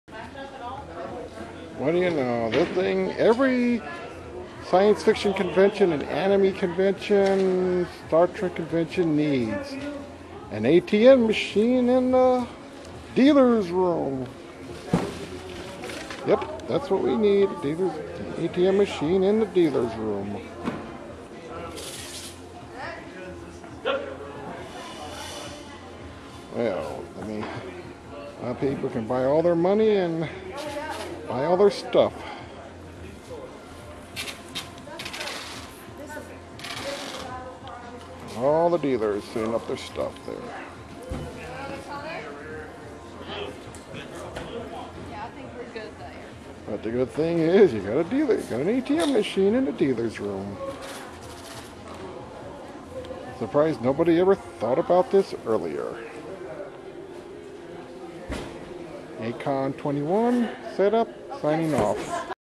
A-KON 21 setup, #5 of 6..Something that should be at any Convention's Dealers Room.
Small clip of setup at A-KON 21 Dealers Room.
Thursday evening.

Here is something that SHOULD BE at all Star Trek Conventions,
Anime Conventions, Sci-Fi Conventions, Media Conventions
Dealers Room.

A device called an ATM Machine.